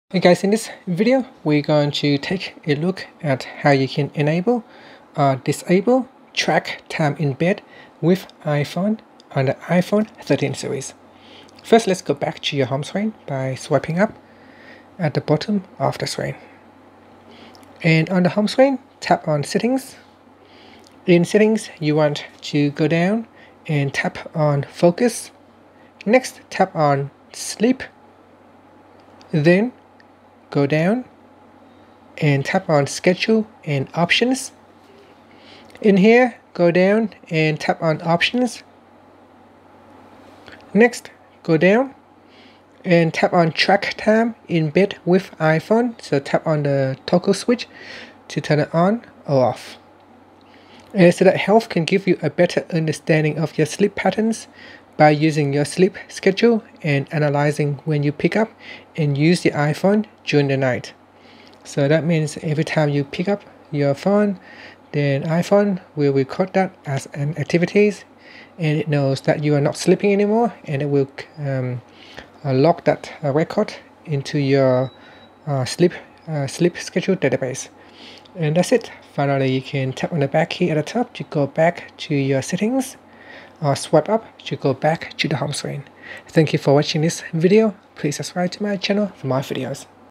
iPhone 13/13 Pro: How to Enable/Disable Track Time in Bed with iPhone
Learn how you can enable or disable track time in bed with iPhone on the iPhone 13 / iPhone 13 Pro.